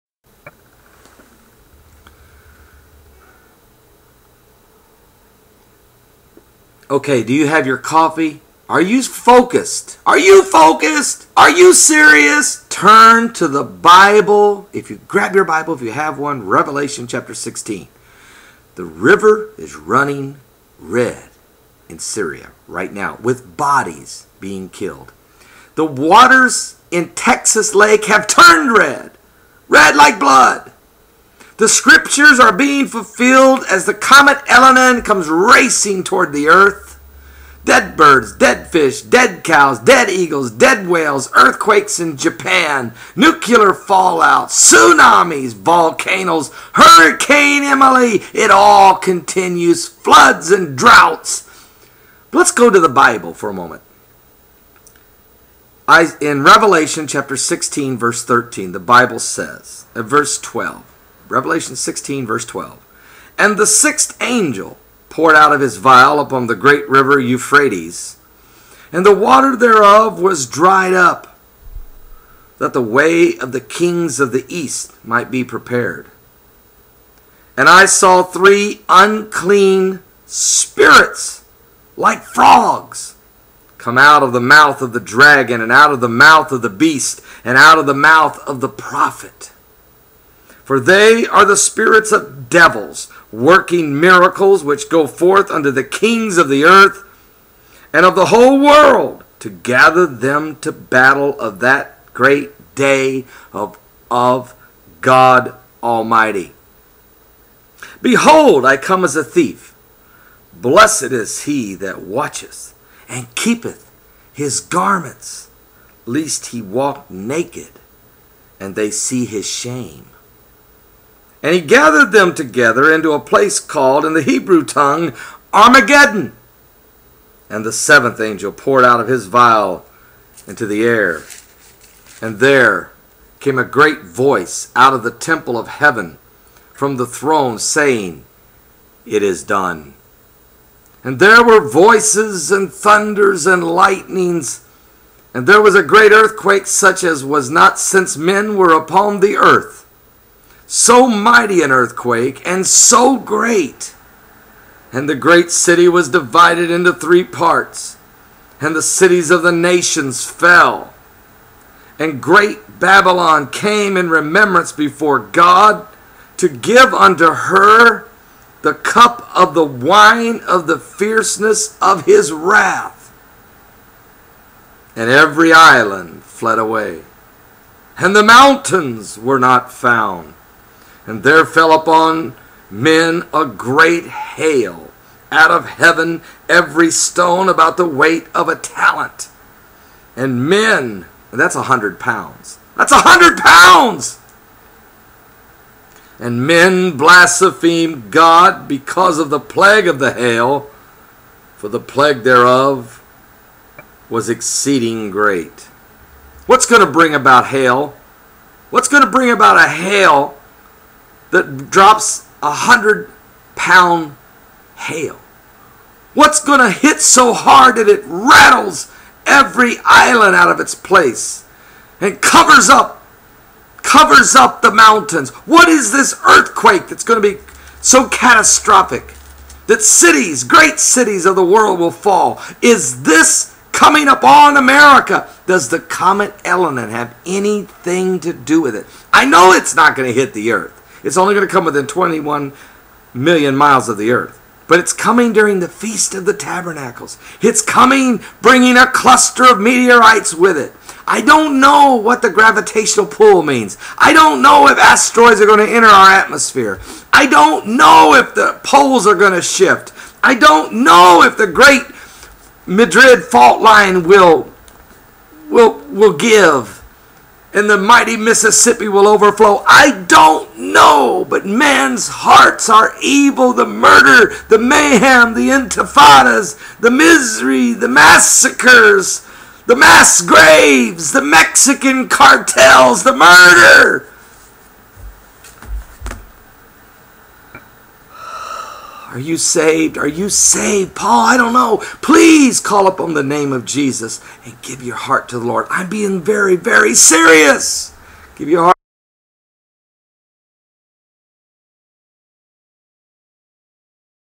Revelation "Coming Apocalypse Is Upon Us" Middle East Riots
Theologian Paul Begley of Indiana reads Revelation 16 and see's the Apocalypse Is Upon Us!! MIddle East riots, blood shed, fear , faith! (some photos were released by different news groups and PB34 don't own them, but used for teaching purpose only)